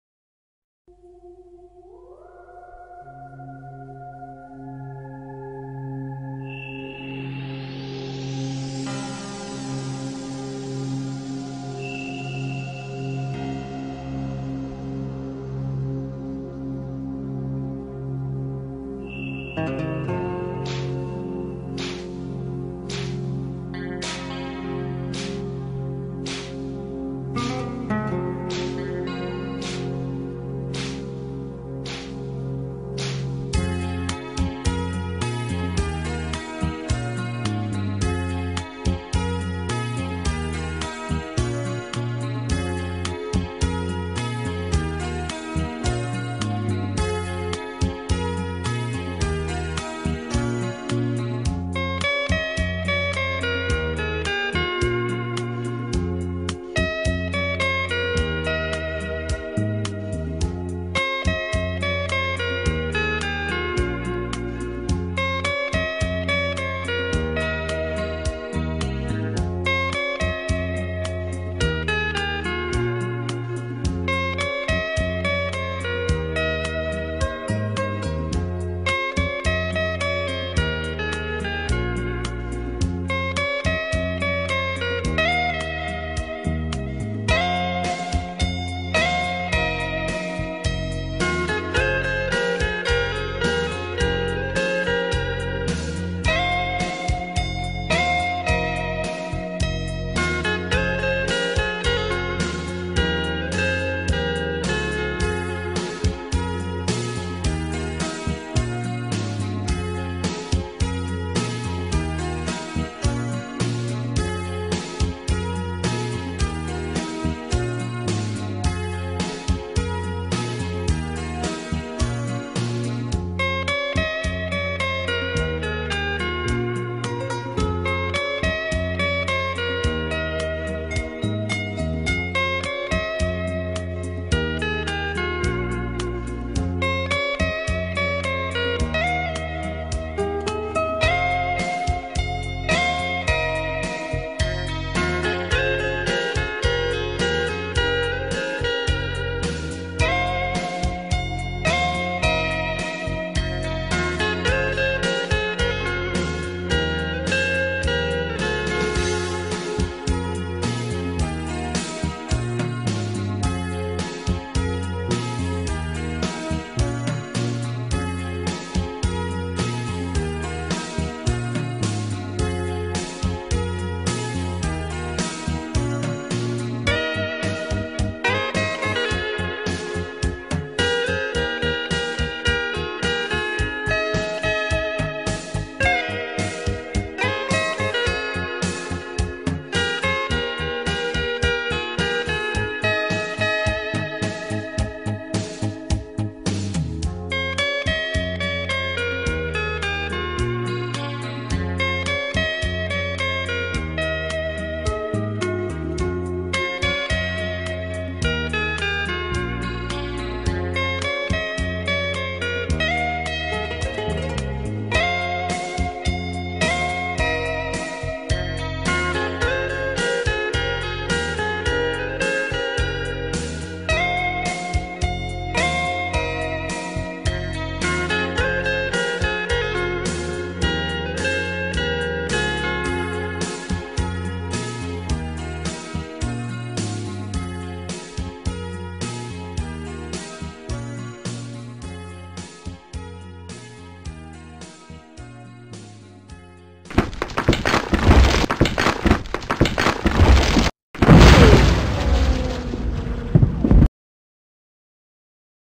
Sony HDR-AX2000
Filmed with the Sony HDR-AX2000 and a tripod testing dept-of-field and quality of video.